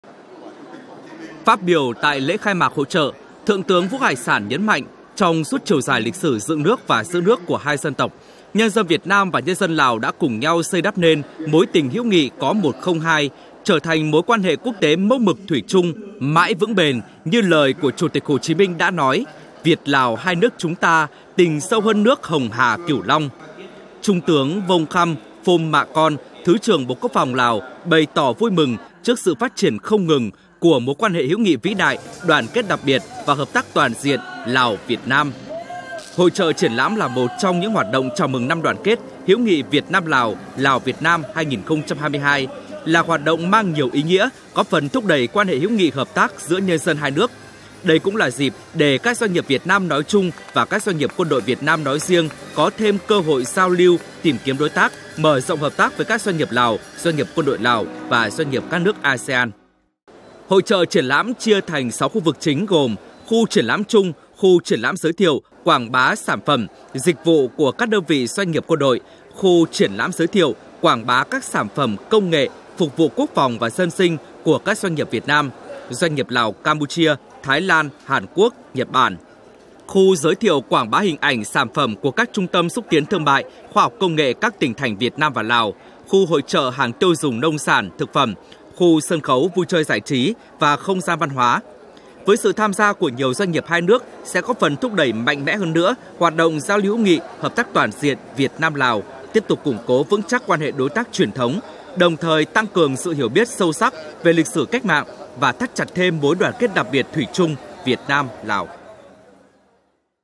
KHAI MẠC HỘI CHỢ TRIỂN LÃM XÚC TIẾN THƯƠNG MẠI VIỆT NAM - LÀO
Ngày 3/11, tại Thủ đô Viêng Chăn (Lào), Bộ Quốc phòng Việt Nam chủ trì phối hợp với Bộ Quốc phòng, Bộ Công Thương Lào tổ chức Lễ khai mạc Hội chợ triển lãm xúc tiến thương mại Việt Nam - Lào với chủ đề “Chào mừng năm đoàn kết, hữu nghị Việt Nam - Lào 2022”. Đồng chủ trì lễ khai mạc có Thượng tướng Vũ Hải Sản-  Ủy viên Trung ương Đảng, Thứ trưởng Bộ Quốc phòng Việt Nam và; Trung tướng Vông Khăm Phôm Mạ Kon-  Ủy viên Trung ương Đảng, Thứ trưởng Bộ Quốc phòng Lào. Tham dự có Thượng tướng Phùng Sĩ Tấn- Phó Tổng Tham mưu trưởng Quân đội nhân dân Việt Nam, Trưởng ban Tổ chức Hội chợ triển lãm.  Trung tướng Vo La San Vi Lat Đa Vông-  Phó Chủ nhiệm Tổng cục Chính trị Quân đội nhân dân Lào. Đồng chí Nguyễn Bá Hùng- Đại sứ Việt Nam tại Lào; đồng chí Trần Văn Tùng-  Thứ trưởng Bộ Khoa học và Công nghệ Việt Nam.